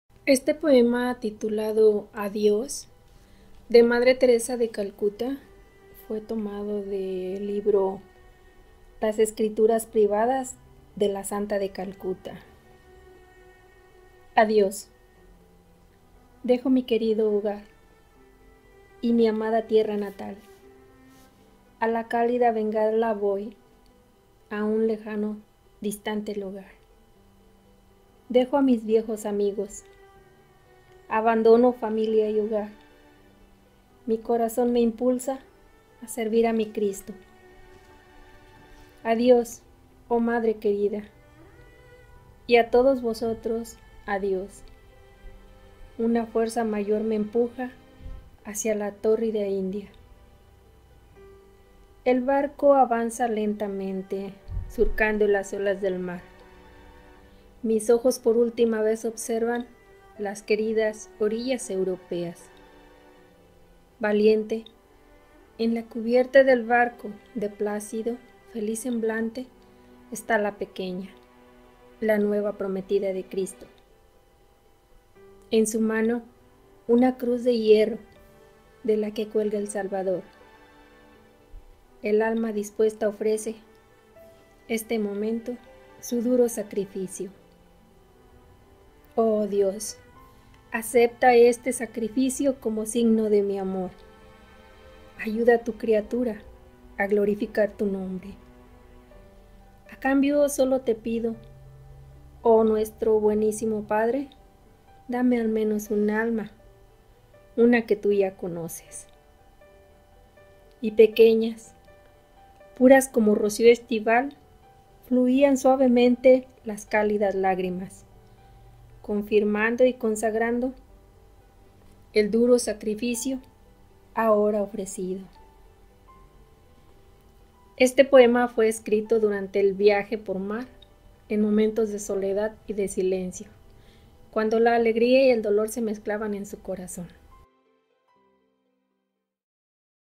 Poema -ADIÓS- Madre Teresa de Calcuta
«Pon tu mano en Su mano [la de Jesús] y camina sola con Él. Camina hacia delante, porque si miras atrás volverás.»(1) Estas palabras de despedida de su madre se grabaron en el corazón de Gonxha Agnes Bojaxhiu, la futura Madre Teresa, al dejar su hogar en Skopje para comenzar su vida como misionera a los dieciocho años. El 26 de septiembre de 1928 viajó a Irlanda para entraren el Instituto de la Bienaventurada Virgen María (las Hermanas de Loreto), una congregación de religiosas, no de clausura, dedicadas principalmente a la educación. Ella había pedido ir a las misiones de Bengala. Semejante aventura exigía gran fe y valentía, ya que ella y su familia sabían bien que «en esa época, cuando los misioneros se iban a las misiones, nunca regresaban».